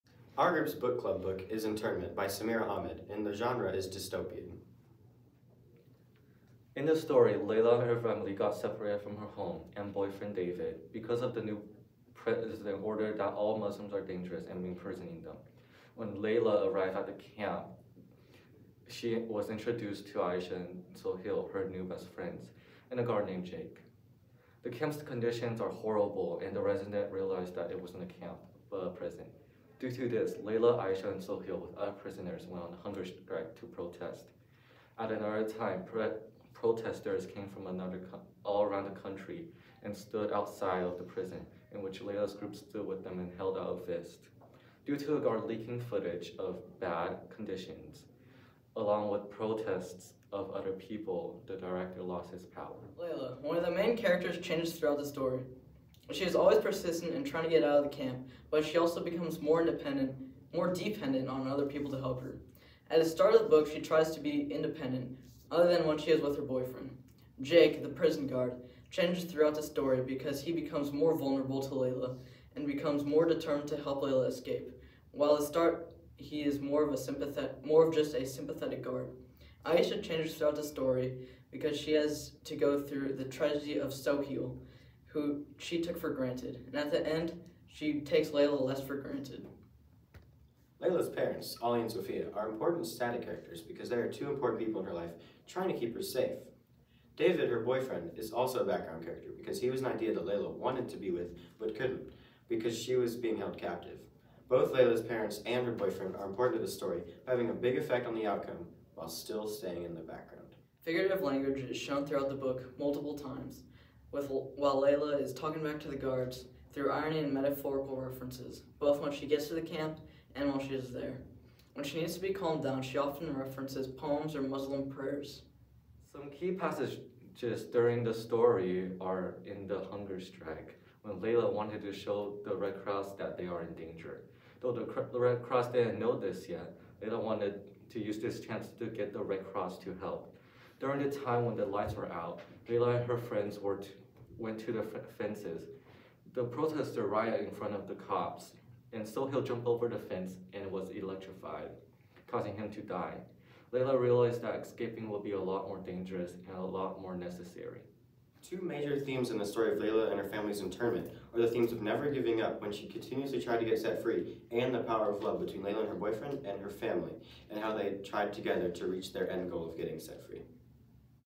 Hour 6 Internment
Group book talk project over Samira Ahmed's young adult novel Internment